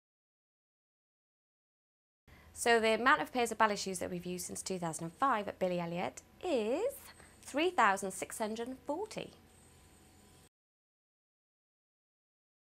Billy Trivia Answer: How many ballet shoes have been used since 2004? | Billy Elliot The Musical
Billy Trivia, with Lauren Adams, Swing Dresser for Billy Elliot. Do you know the answer to Lauren's question? Answer to follow later.